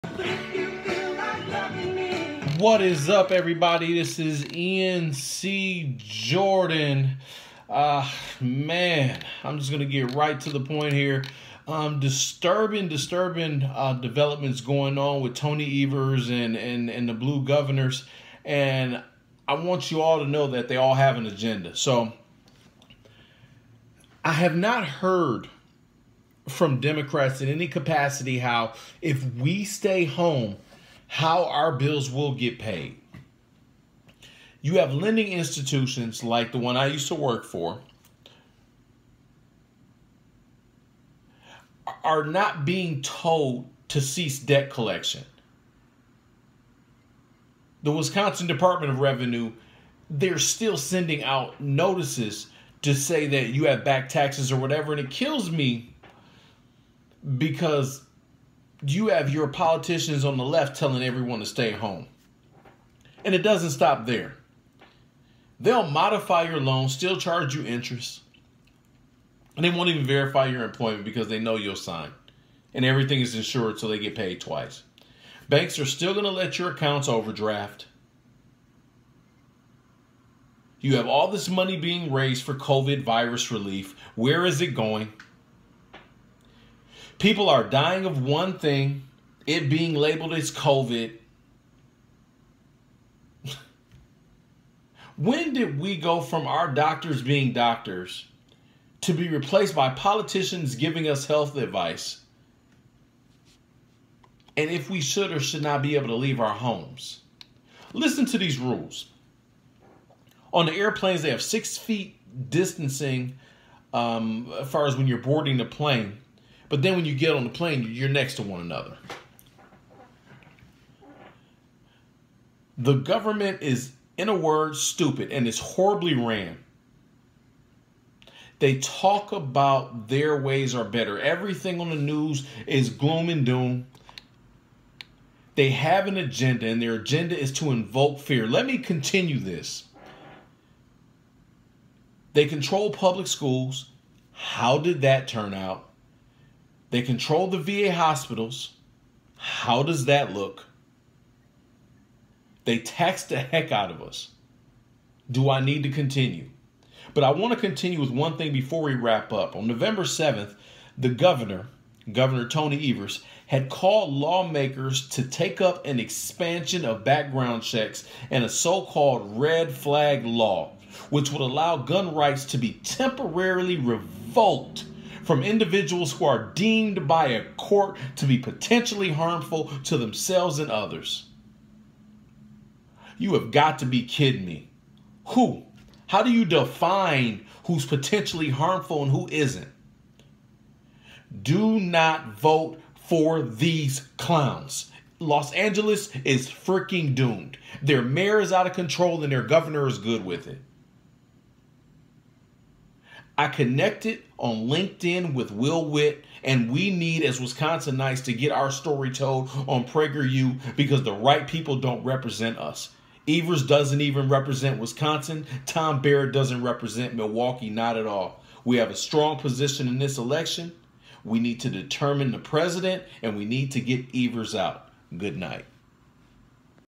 Dont Let Them Fool You:Tony Evers and Blue Governors Have An Agenda: This Will Get Banned On Twitter
I have not heard from the Democrats how if we stay at home how are bills will be paid. The lending institutions like where I worked at Chase are not being told to cease debt collecting. It doesn’t stop there they will modify your loans and still charge interest,without verifying employment.

Banks will still let your account overdraft.
All of this money raised for Covid virus relief. Where is it going ? People dying of one thing then it being labeled as Covid. When did we go from doctors being doctors to be replaced by politicians giving us health advice. And if we should or shouldn’t leave our homes

Listen to these rules, on airplanes 6ft distancing boarding, next to each other on the plane. The government is stupid it is ran horribly. They talk about their ways are better. Everything on the news is gloom and doom.They have an agenda to invoke fear

They control public schools, They control VA hospitals, they tax the heck out of us. Do I need to go on.

Nov 7th The governor had called on lawmakers to take up an expansion of background checks and a so-called "red flag" law, which would allow gun rights to be temporarily revoked from individuals who are deemed by a court to be potentially harmful to themselves or others.

Says who, Do you get what Im saying this EVERS  and the other Govs you dont understand that the right to bear arms is a serious thing. Do Not vote for this man . LA is doomed, their mayor is out of control and the GOV is good with it. I connected with Will Witt, trying to get on Prager U this year our story needs to be told. The right people dont represent us. Evers doesnt represent Wisconsin. Tom Barrett doesn’t represent Milwaukee, not at all.

Wisconsinites have a strong position in this election, we may determine the President, and we need to get Evers out !